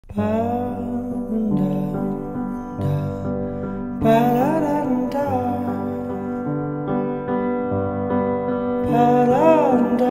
Adding this transition table made a difference
Hi Friends! In todays video I am sharing a table I thrifted. I have been searching for a transition piece to place in between our two living spaces- i love her legs. The spindles are so beautiful .

Thanks a ton for watching !